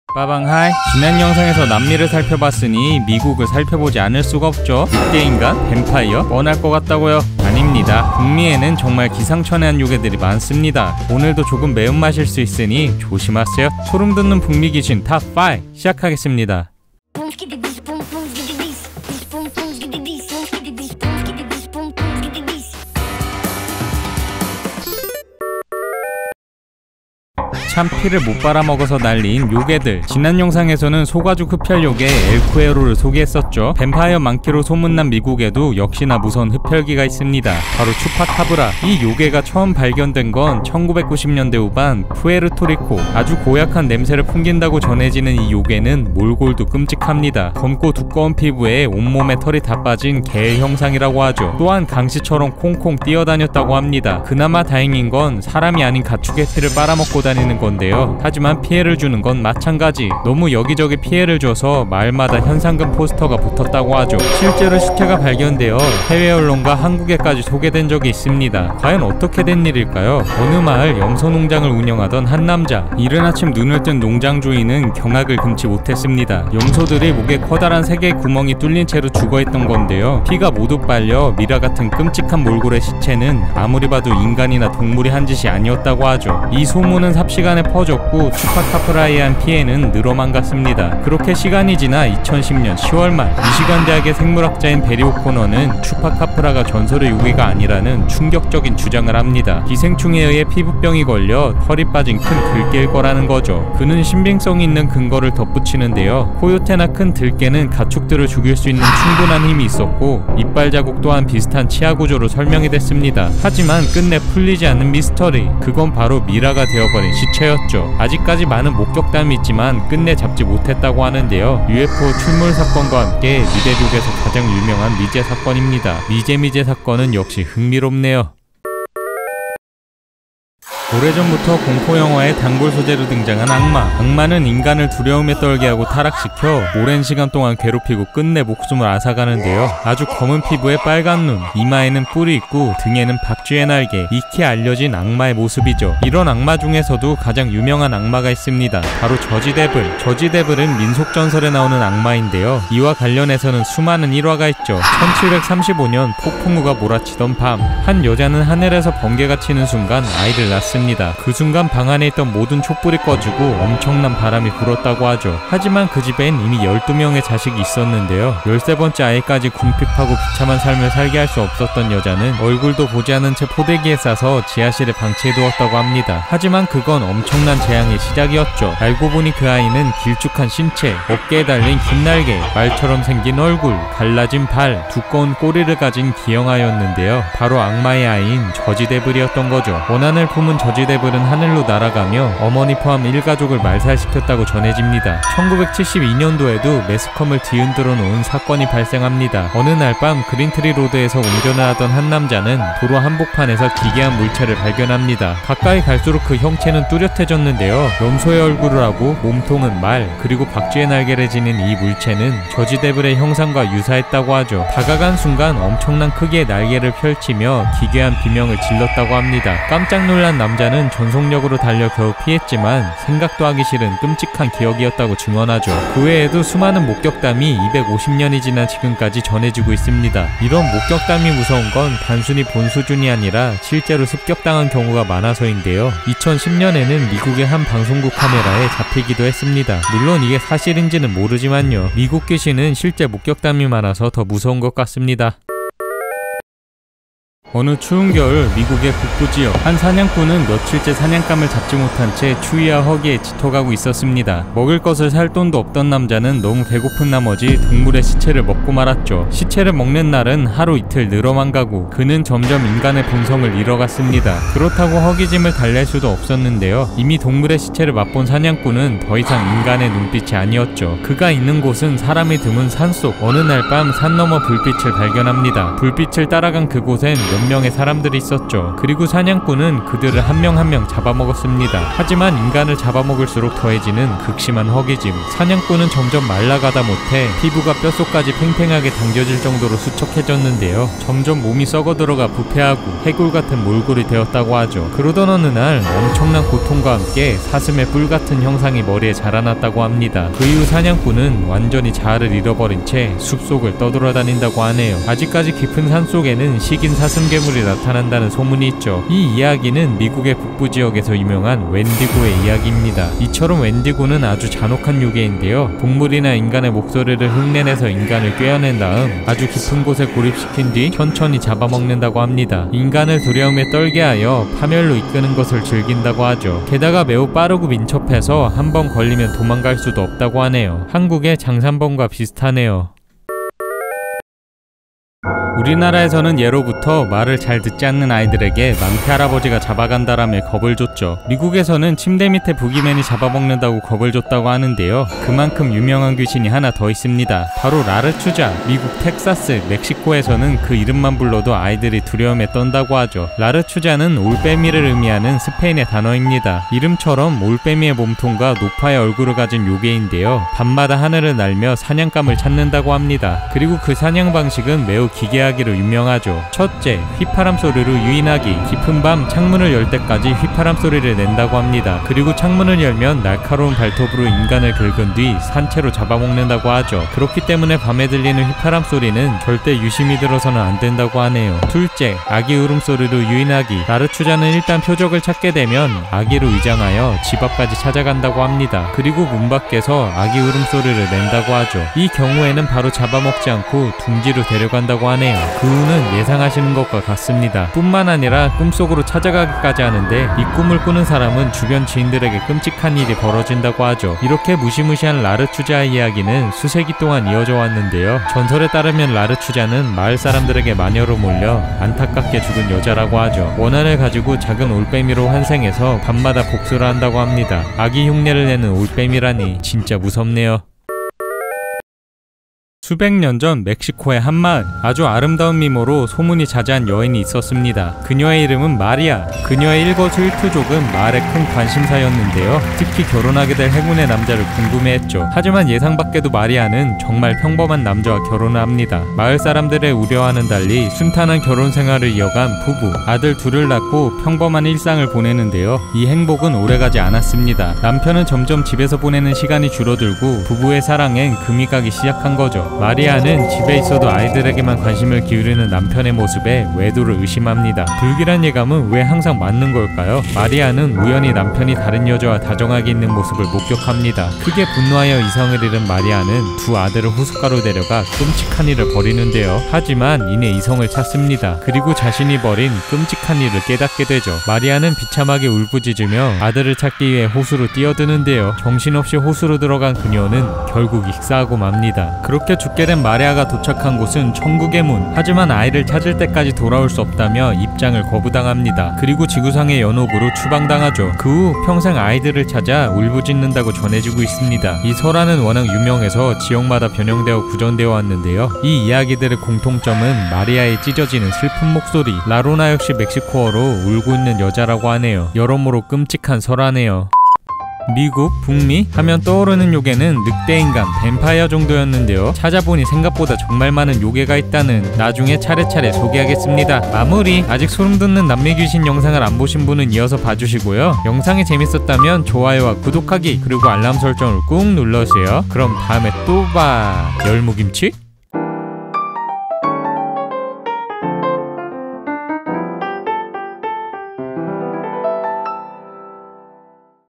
[빠퀴2tv] 소름 돋는 미국 멕시코 귀신 TOP5
미국 혹은 북미 하면 떠오르는 귀신이나 요괴는
뱀파이어, 늑대인간 정도였는데...
역시 땅덩어리가 넓으니 무서운 귀신이 많네요